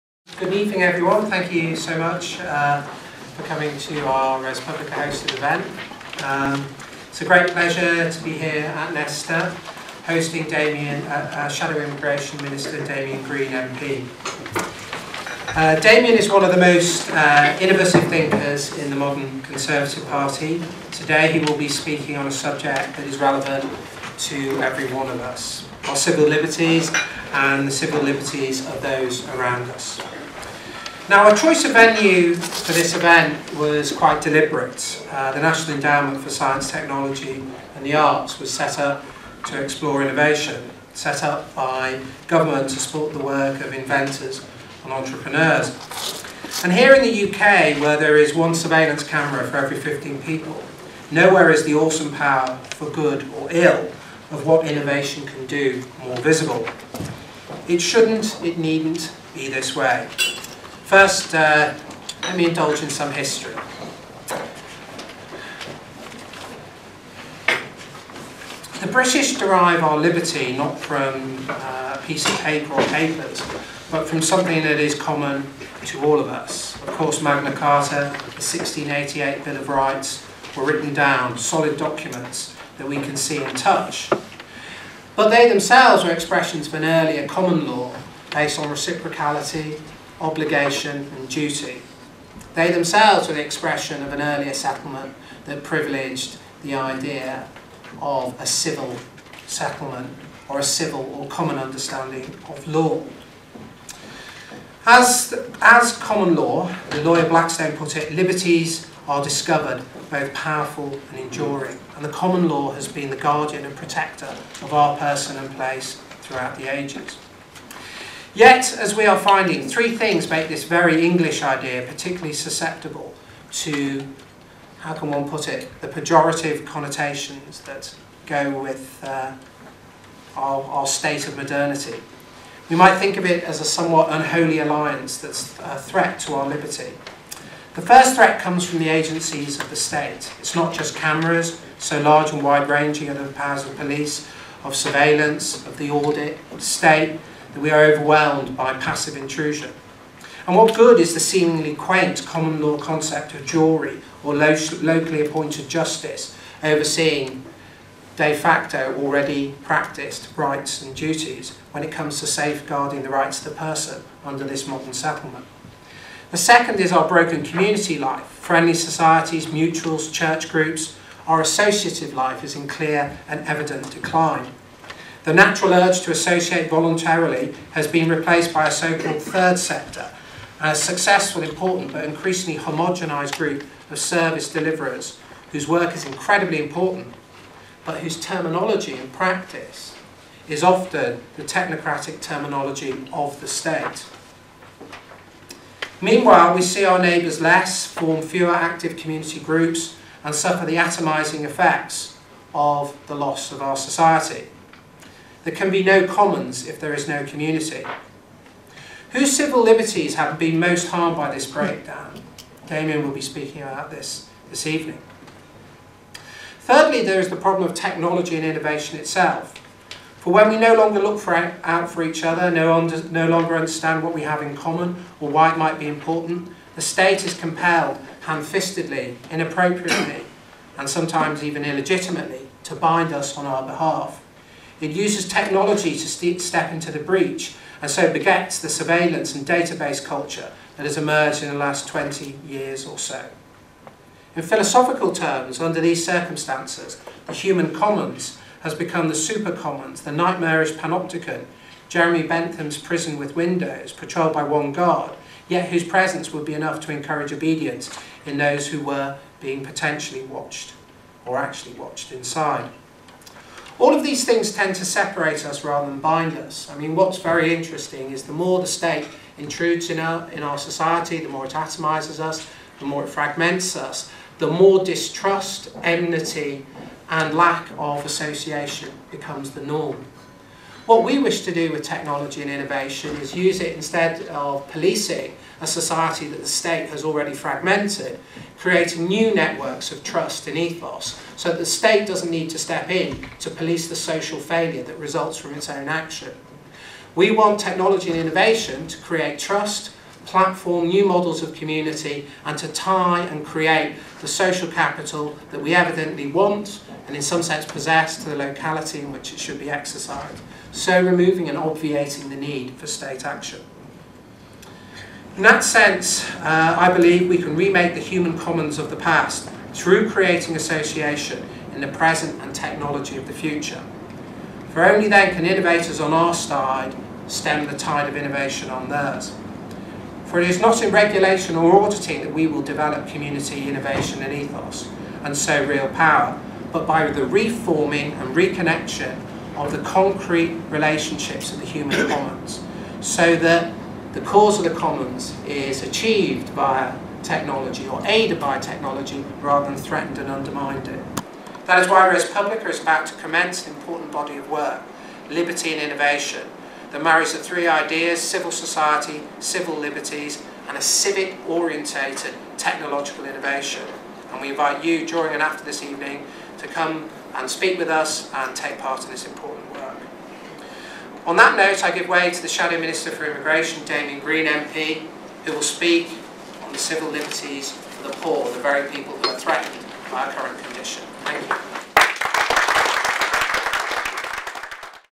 Damian Green MP Speaks To ResPublica - 1 - Phillip Blond's introduction
ResPublica was delighted to host an evening speech by Shadow Immigration Minister, Damian Green MP, on the subject of civil liberties, entitled Poor People Need Civil Liberties Too, at the National Endowment for Science, Technology and the Arts on Tuesday the 2nd of March.